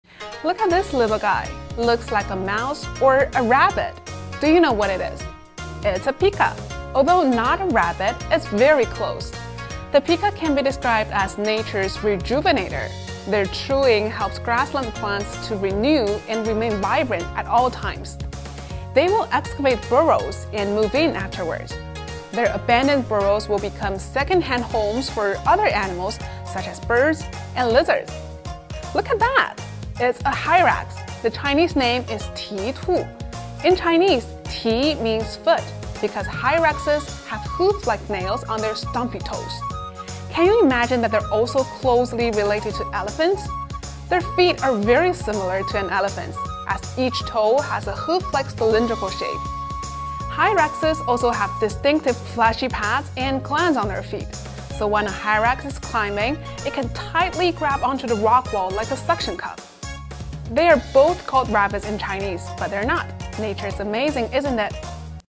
Hopping into Tunian: Some animals have 'rabbit' in Chinese names, but they are not rabbits
Animal names can be confusing in Chinese. Some animals' names might mislead people to believe that they are some kind of rabbit, but actually they are not. Watch this video to find out more.